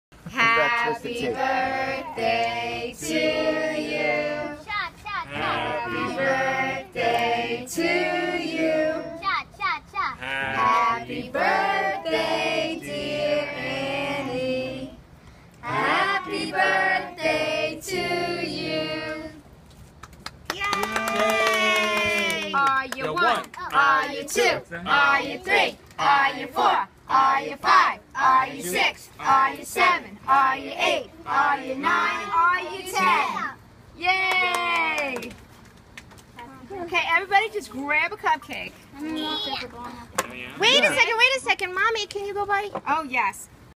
Happy 10th Birthday Annie - 08.16.2015
in the backyard with yummy Hello Kitty cupcakes